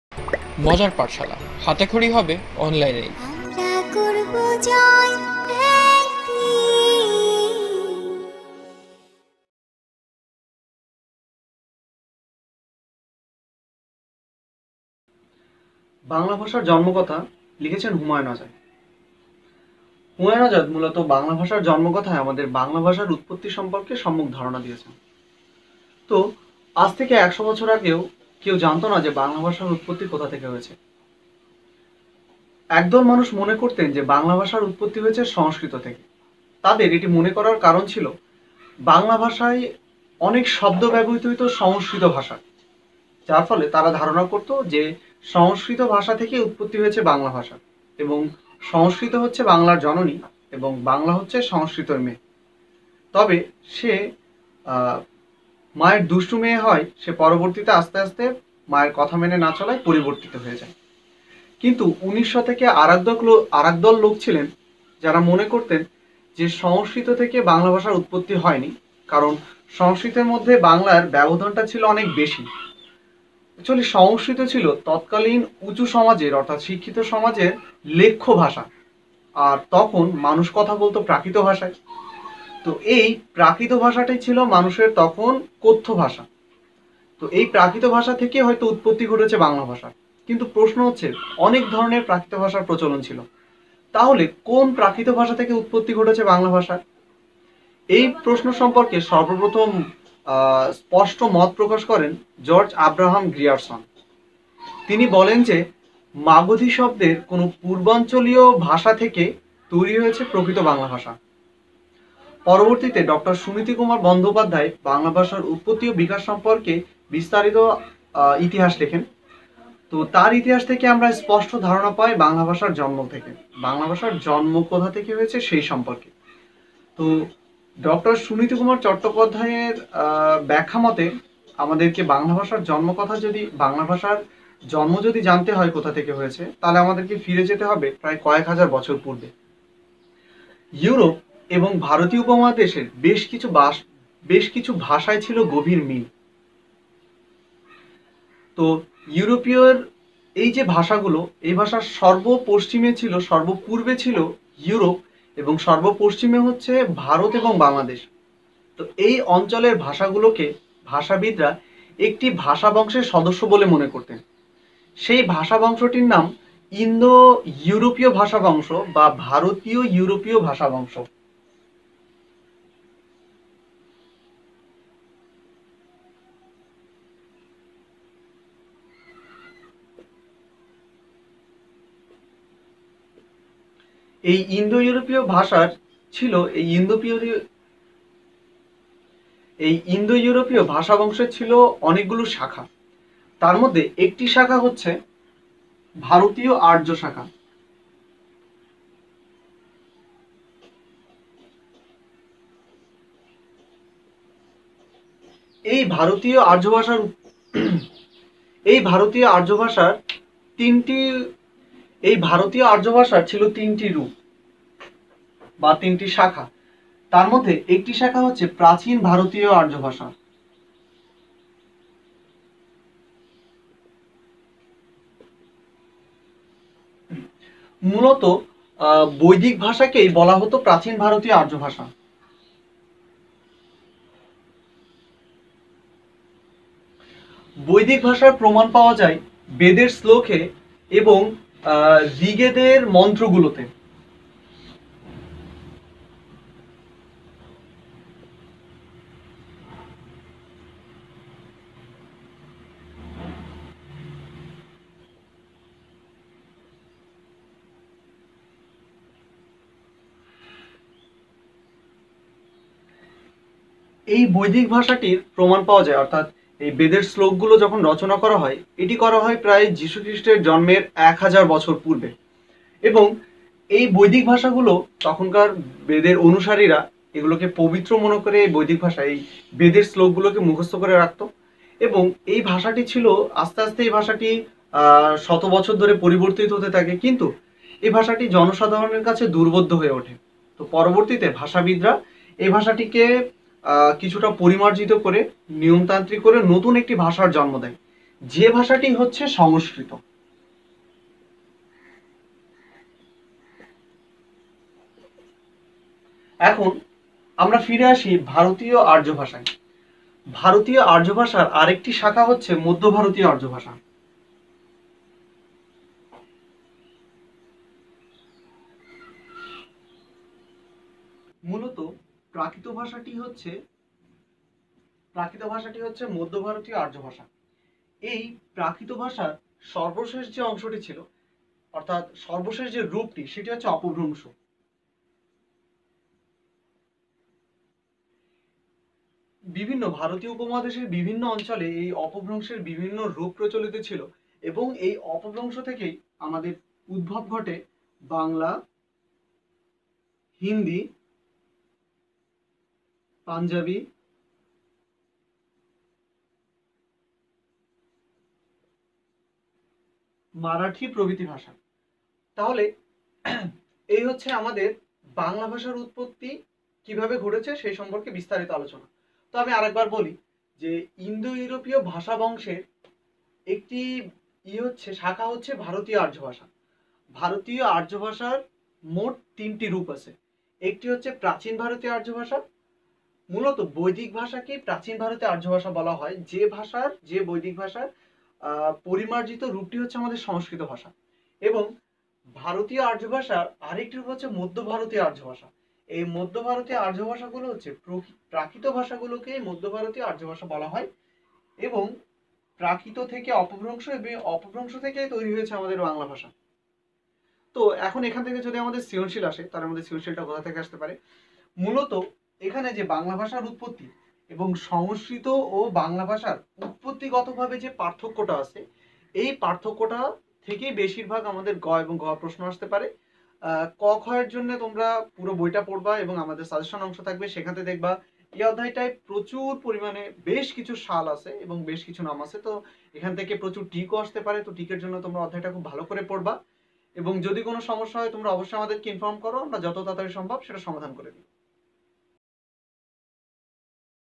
১১। বাংলা ভাষার জন্মকথা। ৮ম শ্রেণী বাংলা
Video Title: Mojar Pathshala
Lecture:  Golpo "বাংলা ভাষার জন্মকথা"
Subject: Bangla
Class: JSC (Class 8)
Lectured by: Abdus Sobur